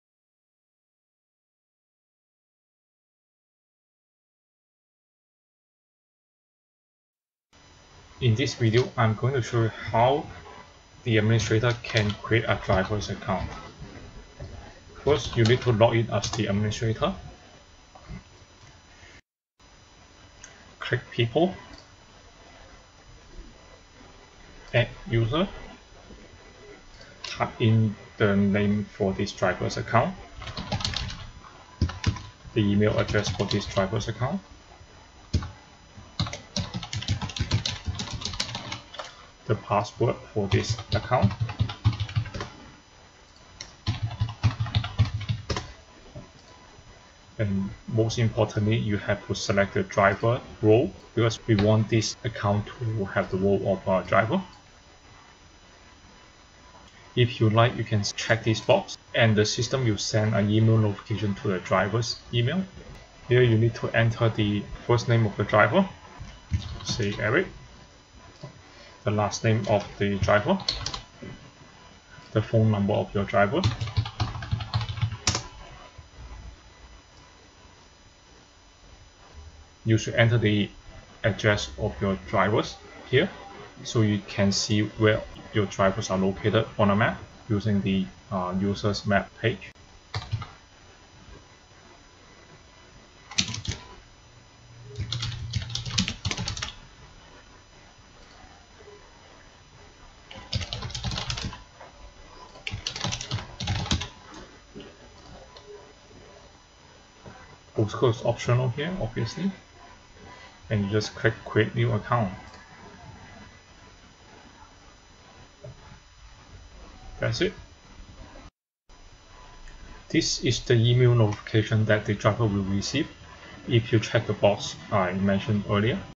LiveParcels Demo: Admin create new driver account.
This video is a demonstration on how the courier administrator creates a new driver account on the LiveParcels system.